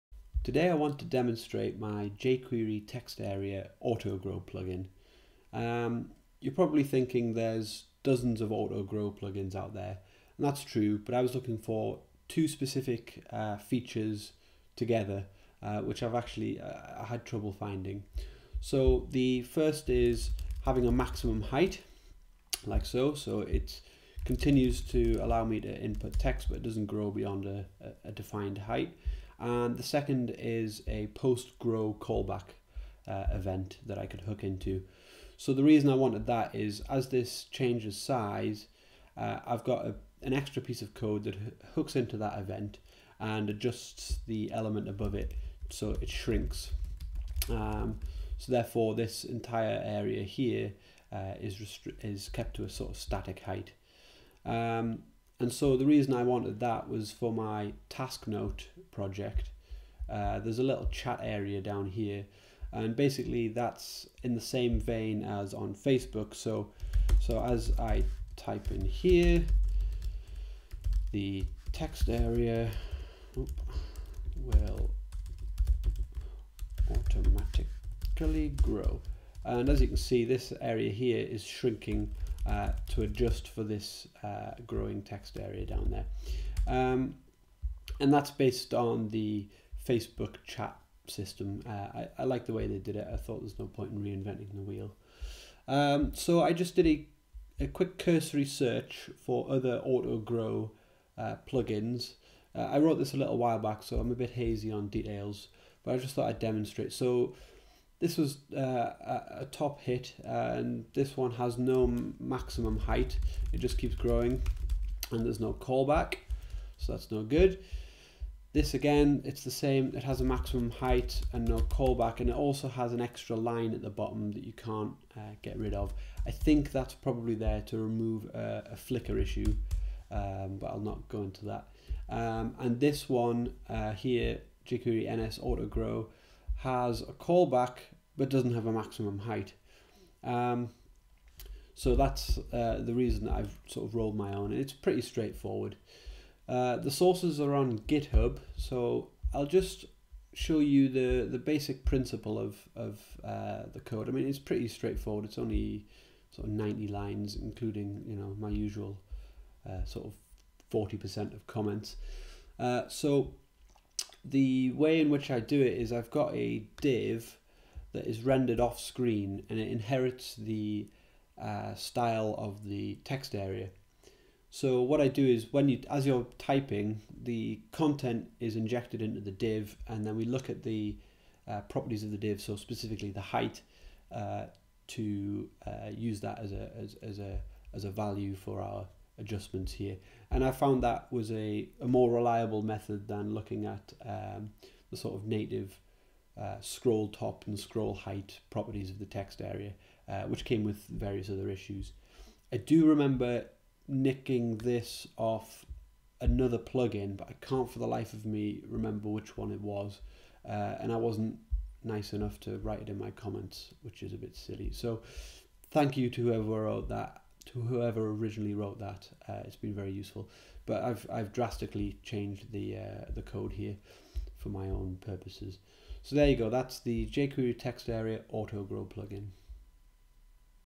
jQuery Textarea AutoGrow
Simple jQuery plugin for automatically growing textareas. Has 3 main features:

* Grows to fit the text content
* Has a maximum height
* Has a callback event